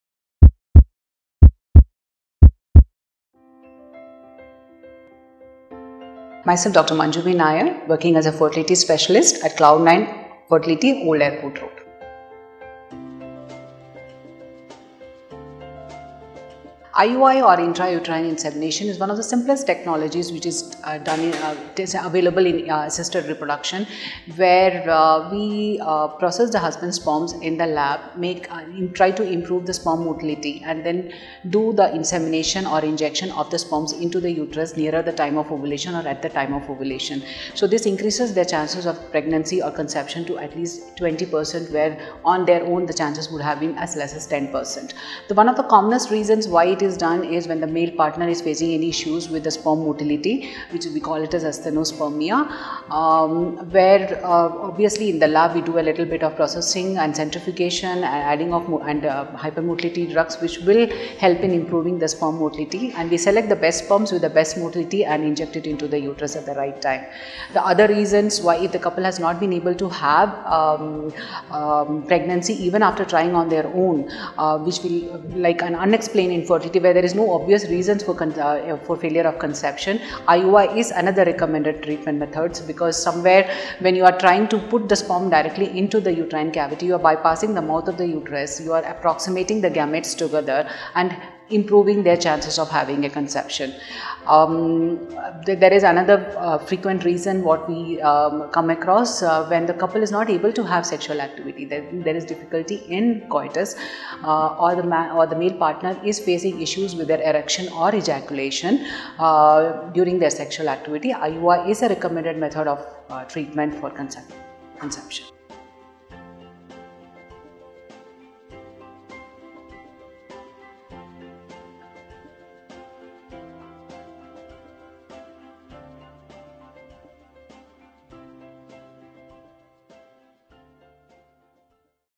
When should a couple go for IUI? - Dr. Manju Nair | Cloudnine Hospitals
IUI or Intrauterine Insemination is a fertility treatment that involves placing sperm inside a woman’s uterus to facilitate fertilization. The goal of IUI is to increase the number of sperm that reach the Fallopian tubes and subsequently increase the chance of fertilization. This is done when the male sperm quality is not strong, unexplained infertility and when there is difficulty in ejaculation, erection or inability to have coitus.